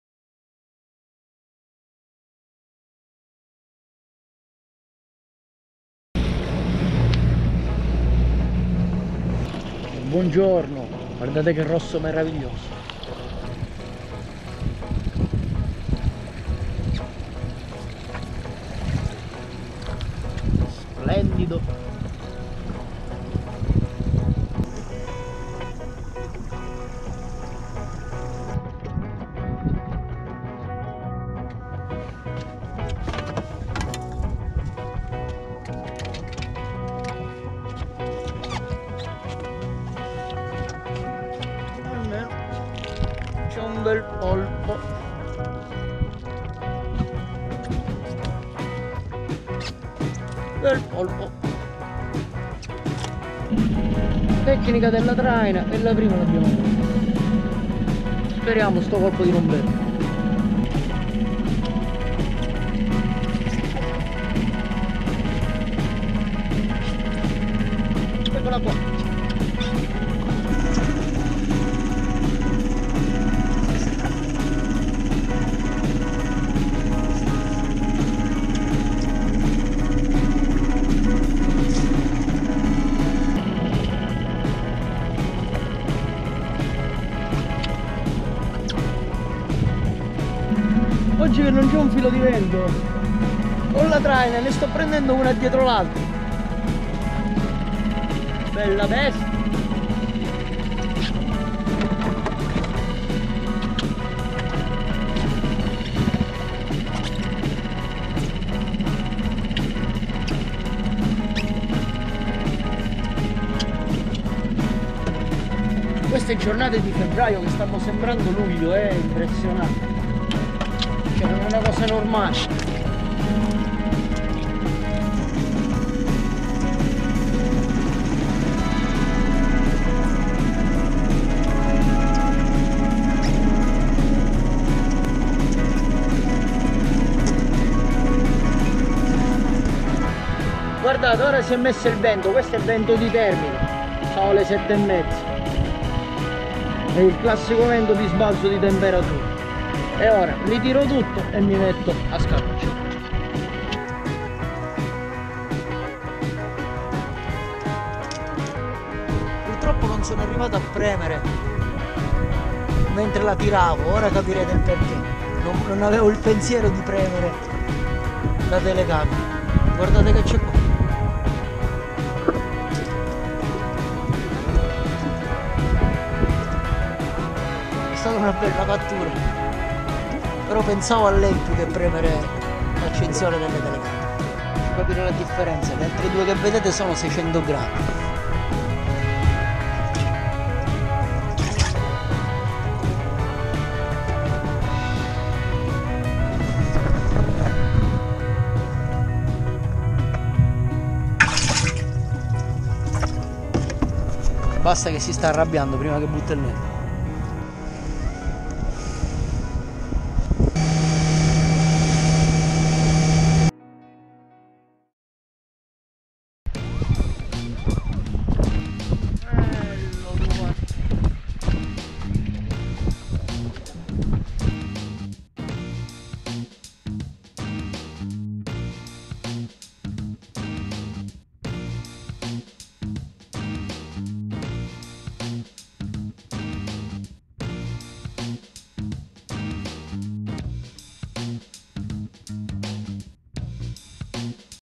Seppie & Polpo - Pesca a traina e a scarroccio ai cefalopodi
Una bella mattina a pesca di cefalopodi. Prima un bel polpo seguito da delle seppie prese con la tecnica della traina, per poi finire in bellezza con una grossa seppie di oltre un kg presa a scarroccio
Pesca sportiva nel mare di sicilia - Fishing Experience Catania - Saltwater